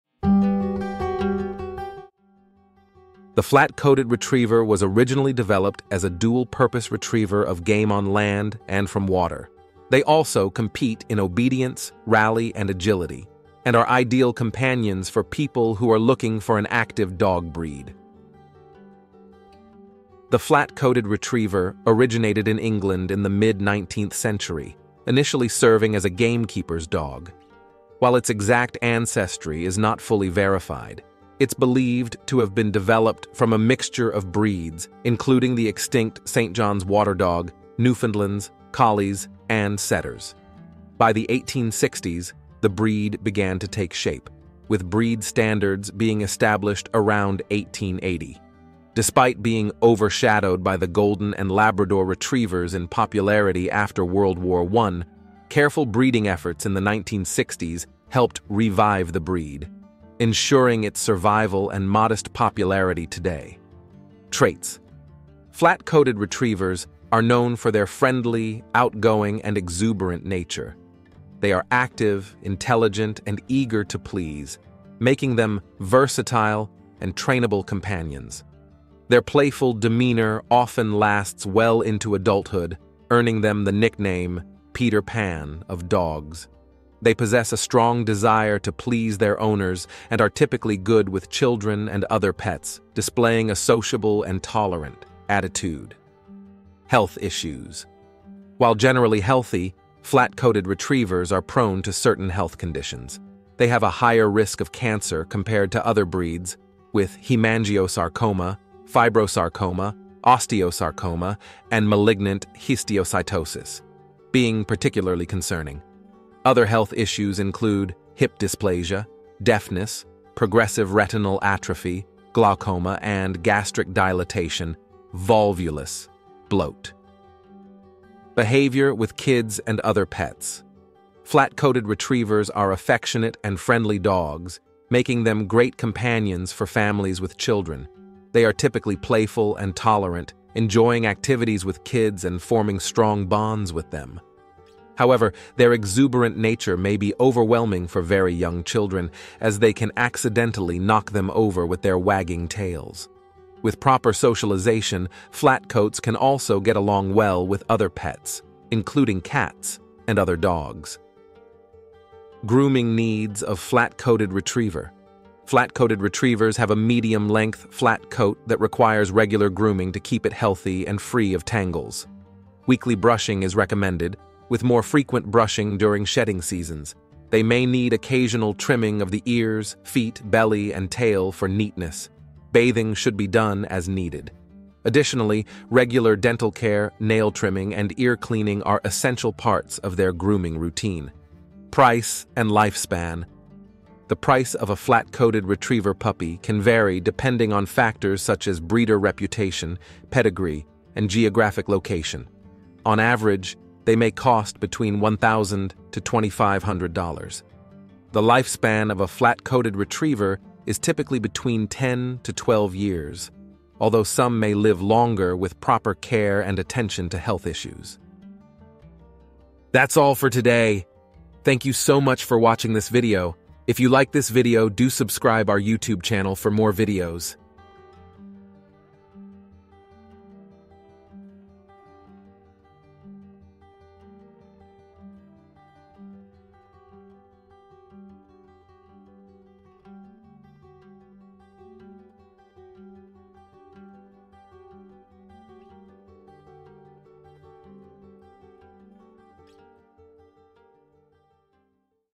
Flat Coated Retriever Breed Profile History - Price - Traits - Grooming Needs - Lifespan
In this video we have covered these topics in details :
Flat Coated Retriever Breed Profile
Flat Coated Retriever History
Flat Coated Retriever Traits
Flat Coated Retriever Health issues:
Flat Coated Retriever Behaviour with kids and other pets:
Flat Coated Retriever Grooming needs:
Flat Coated Retriever Price
Flat Coated Retriever Lifespan